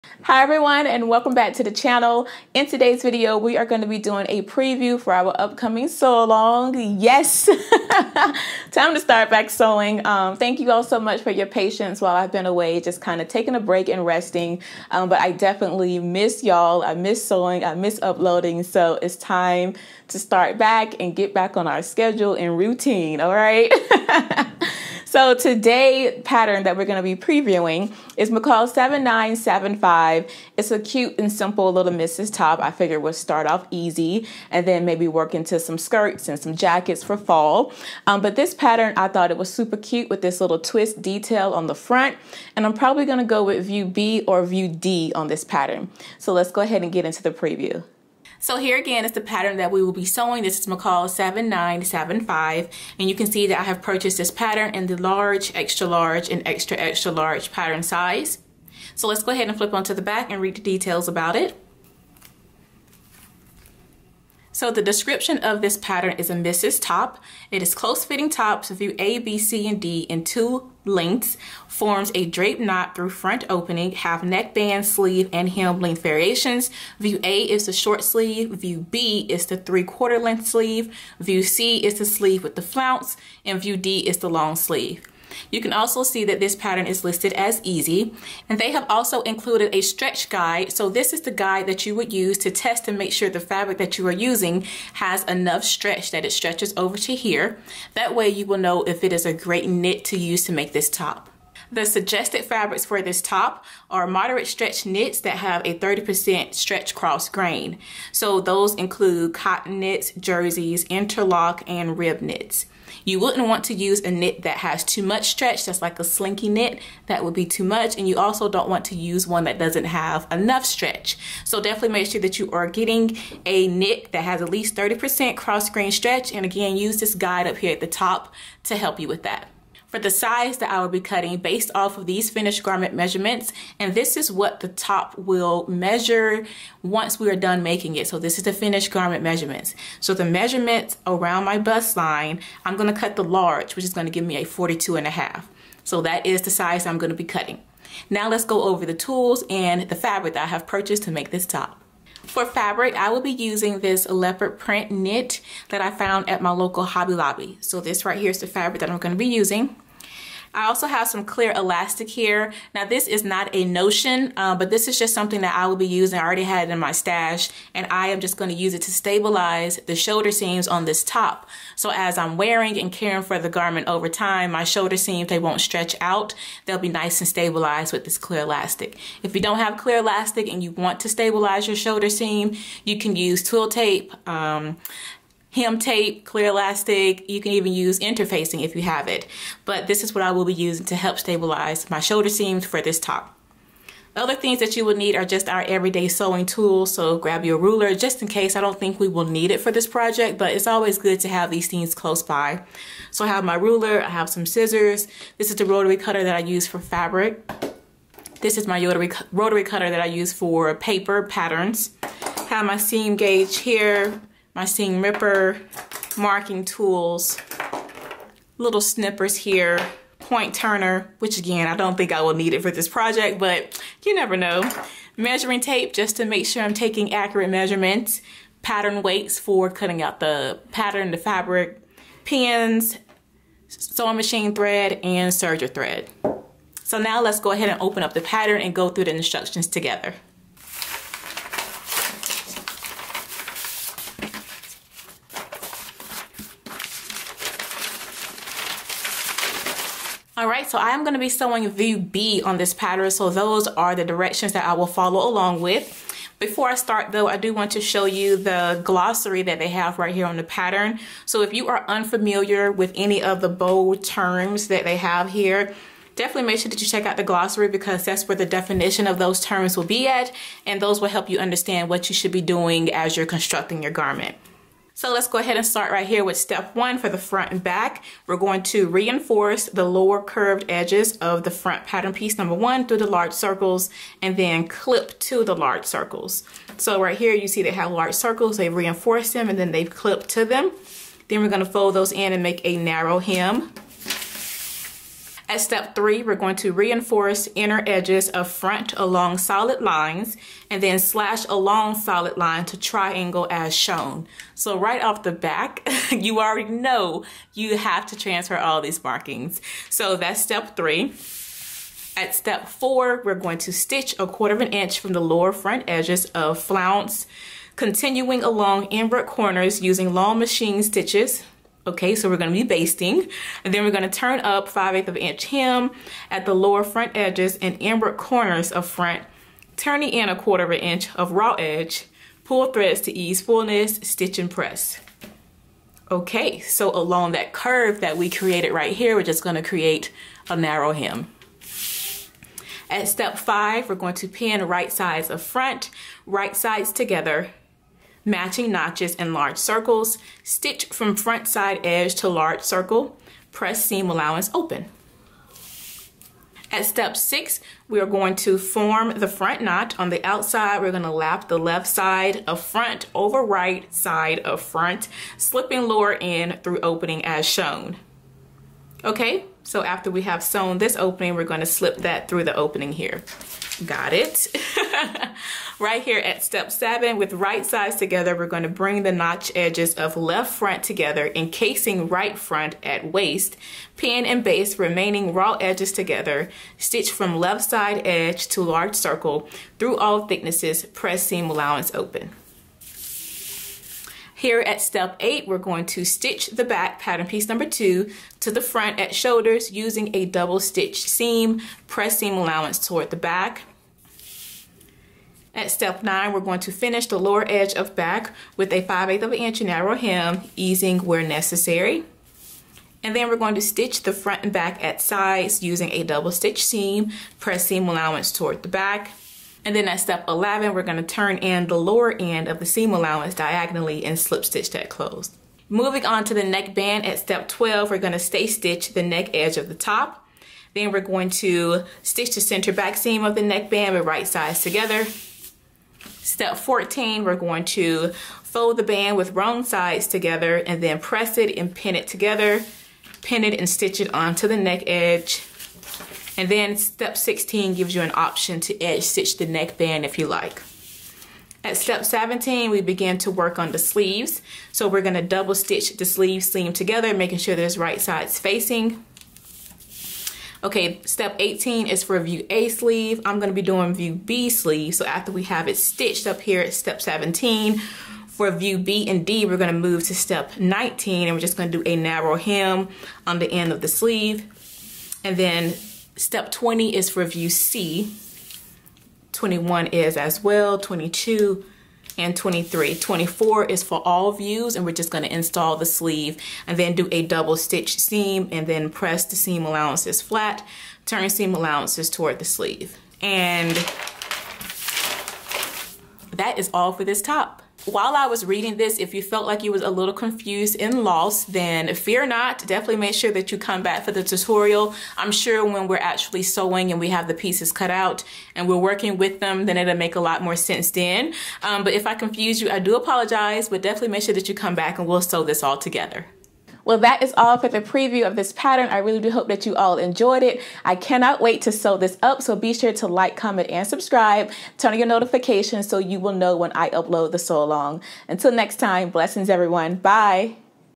PATTERN PREVIEW: MCCALLS 7975
Hey everybody! Welcome back to the channel! Today I am doing a pattern preview for our next sew along McCalls 7975! This is a Misses Top with a front twist detail! Hope you enjoy the video!

Pattern: McCalls 7975